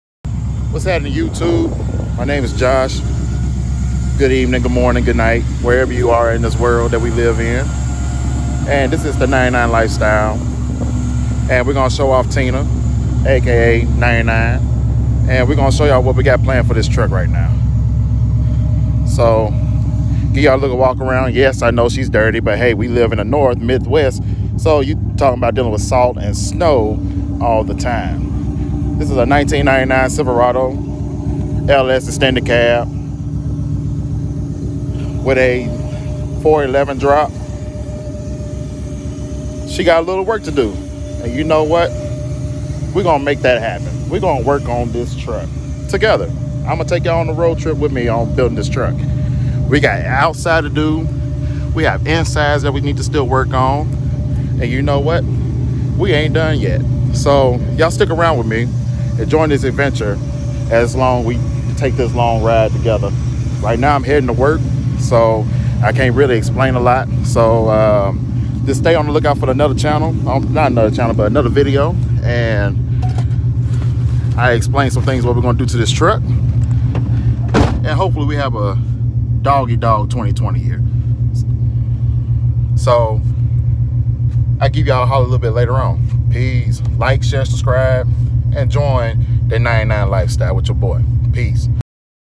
WELCOME TO THE 99 LIFESTYLE (quick vid no cuts!!)
Welcome to the channel first quick vid! Showing raw footage of the truck and just introducing ourselves to the world!